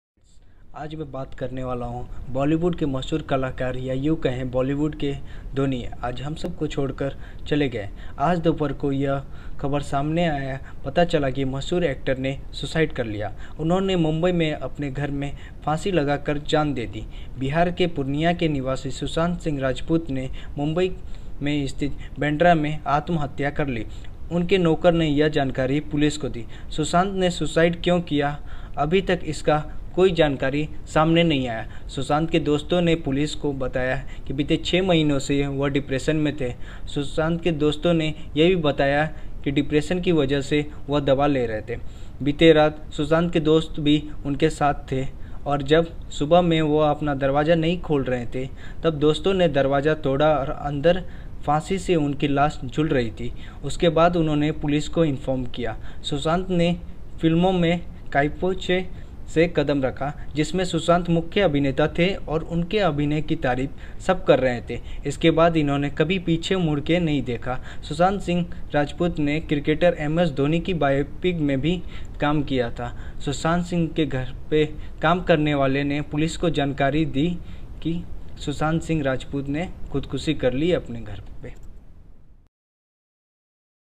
Rip Sushant Singh Rajput😭
This is video about “Rip😭 Sushant singh Rajput (Death)

This video is also about the life of Sushant singh Rajput who is not with us any more..he will be missed by his fans always..Sushant singh rajput was Indian bollywood actor , dancer ,television personality..he started his career with television serials

Note : This video is made on my internet research